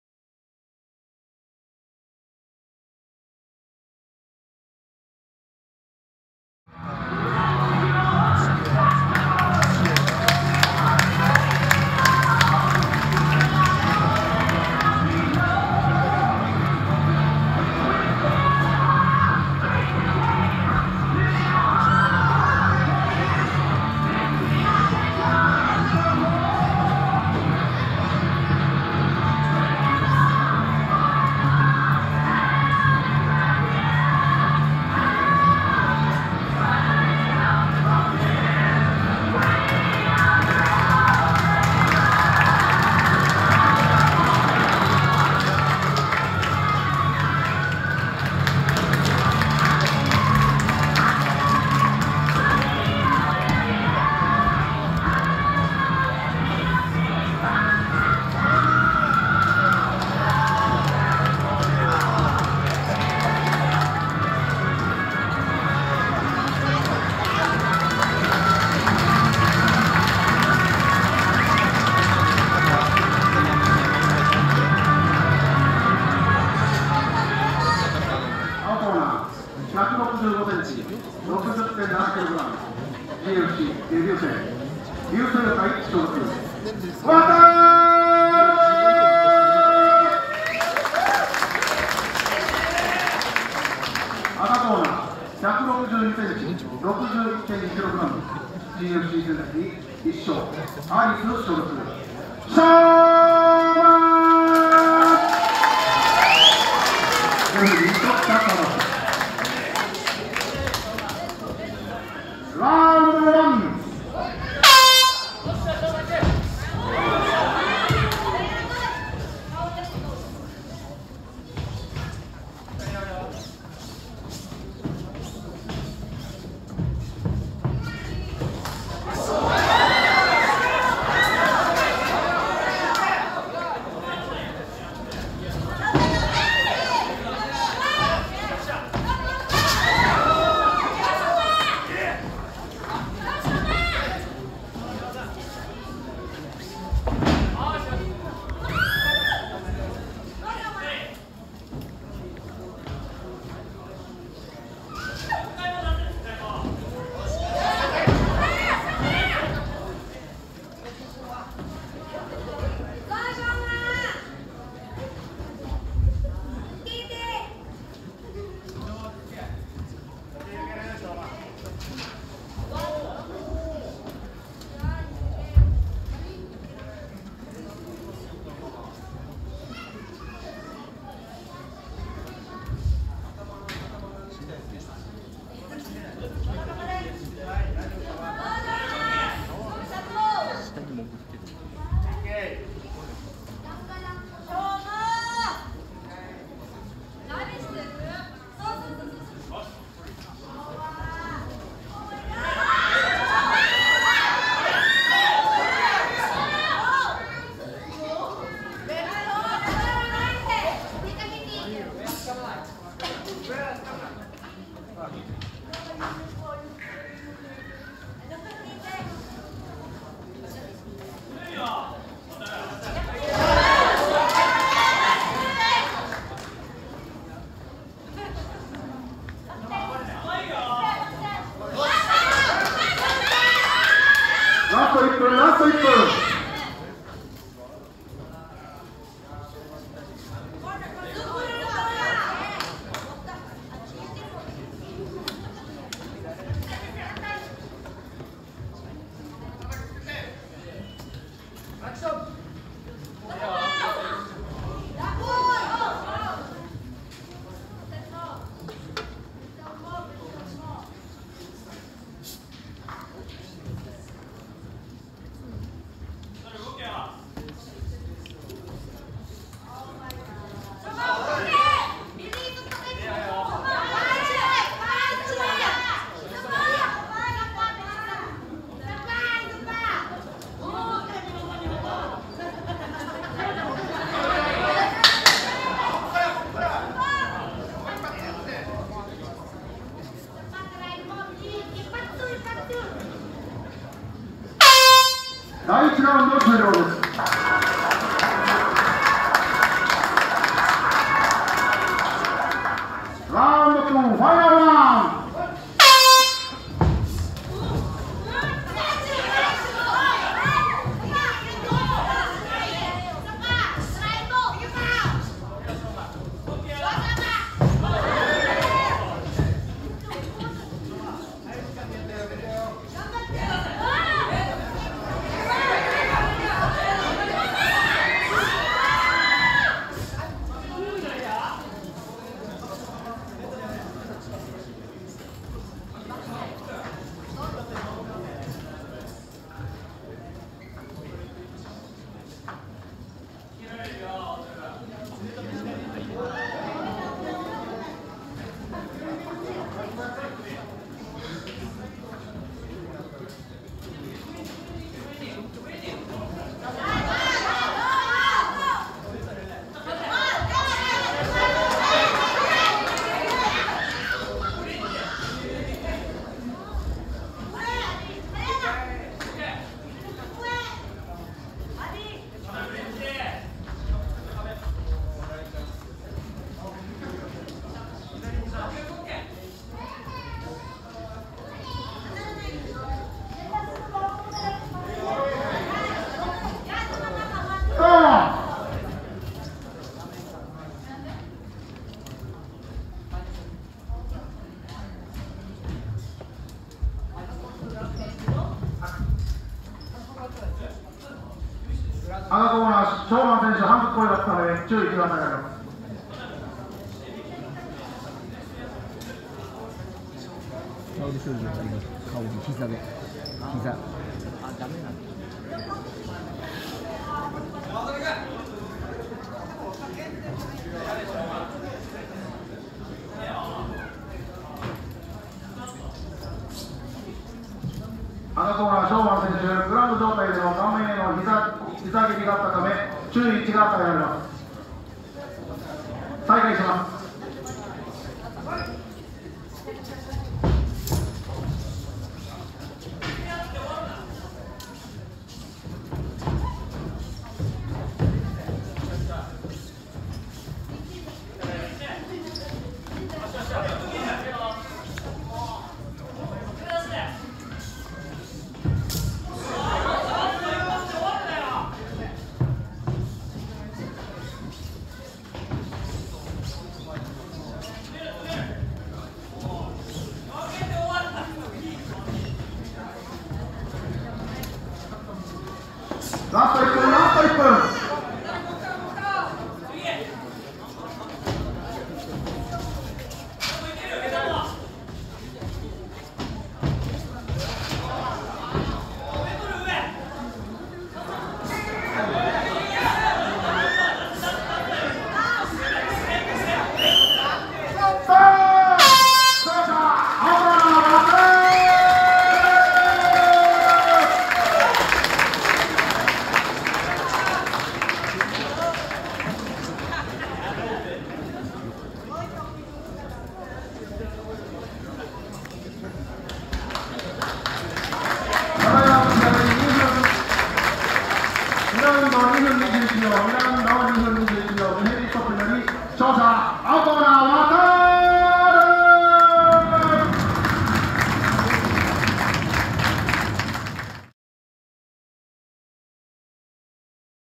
沖縄格闘技大会【GFC】vol.12第4試合
2019/06/16(日)
【GFC】vol.12
in南城市大里環境改善センター
第4試合MMAルール
SHOW☆MAN vs WATARU